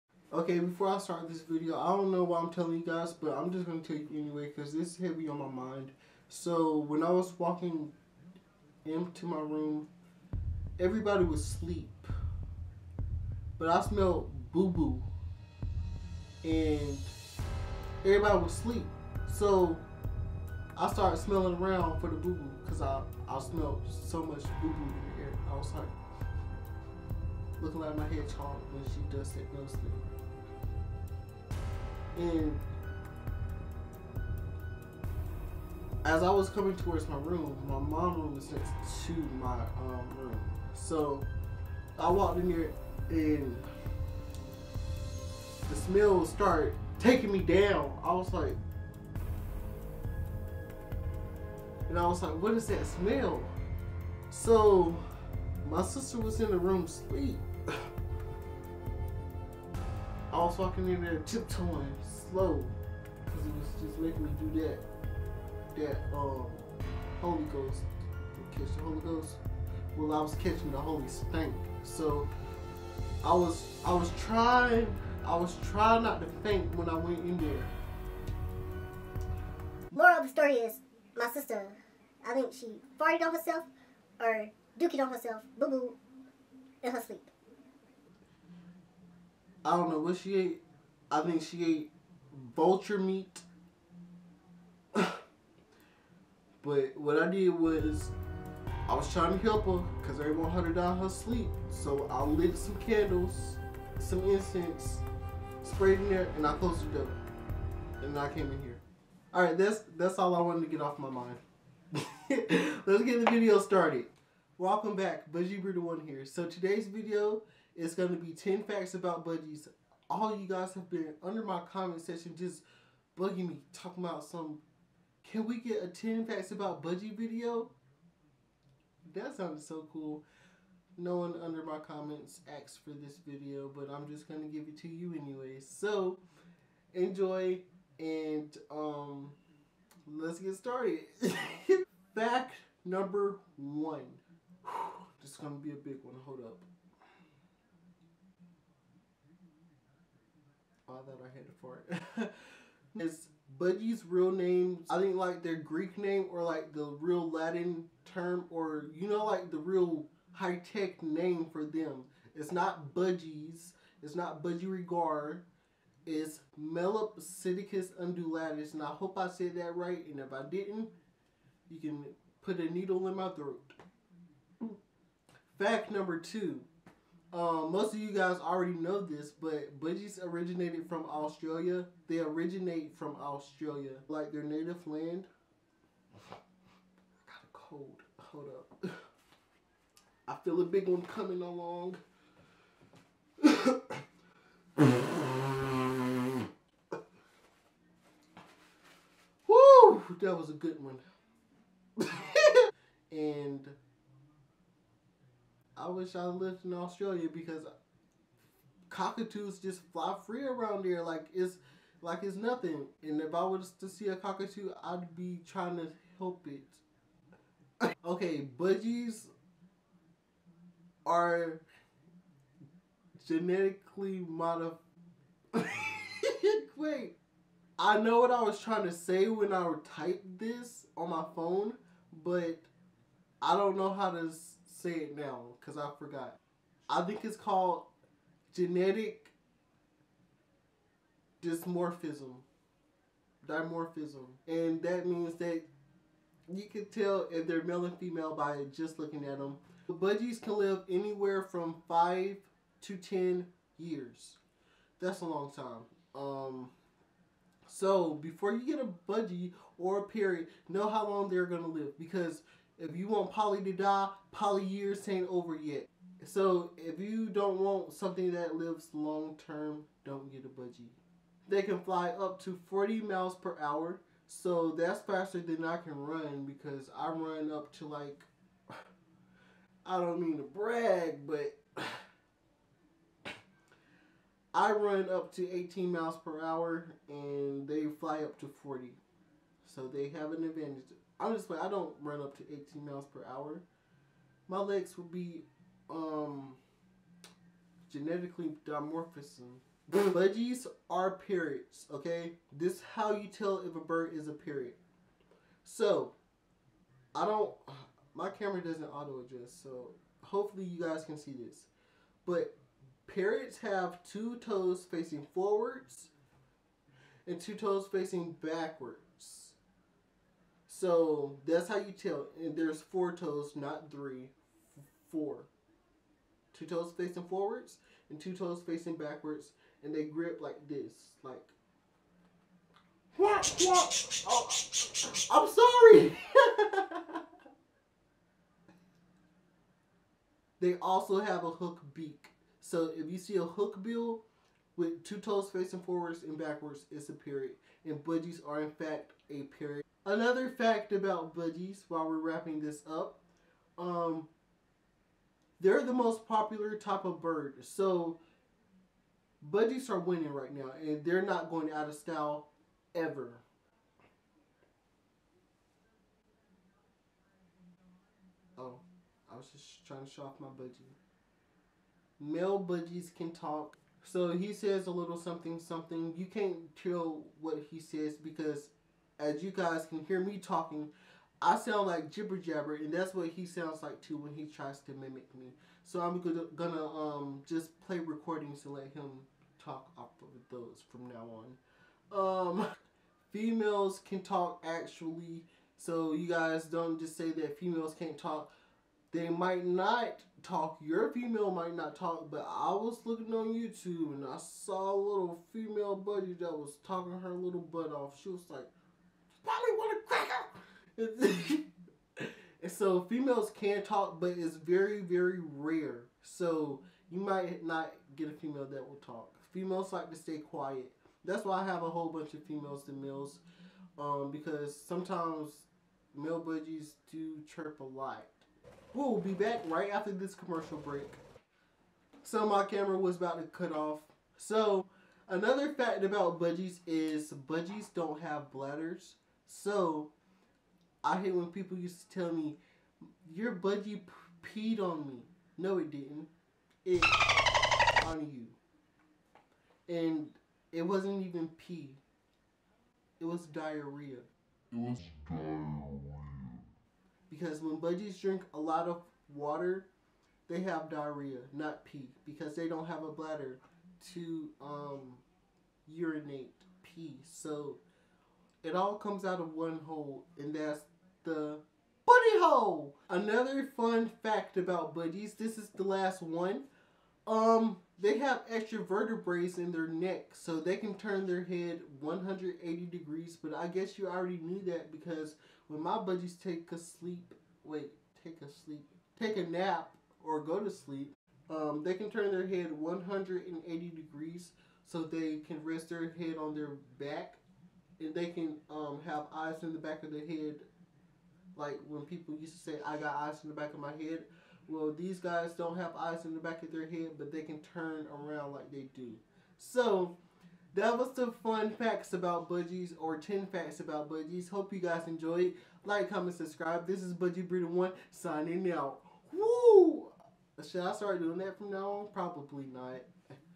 10 Interesting Facts About Budgies
10 facts about budgies,  hope you'll enjoy.
 I didn't play background music this time, hopefully you guys can hear me.